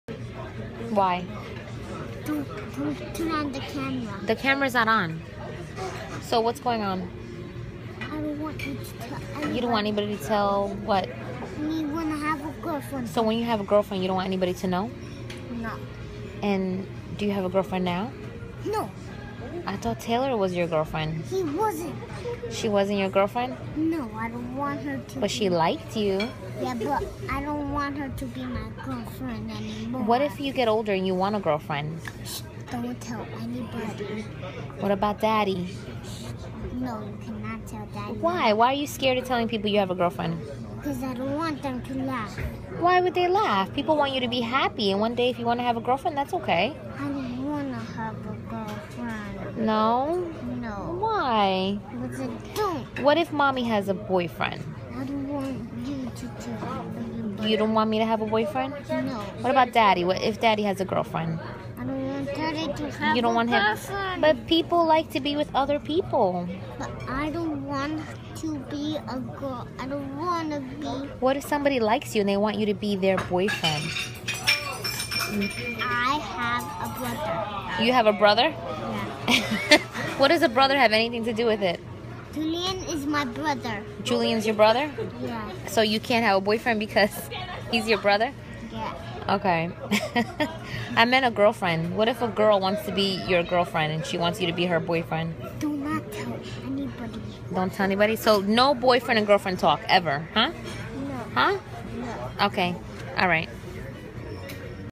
A 5 year old's perspective on Relationships...
My son wants nothing but discretion when it comes to relationships.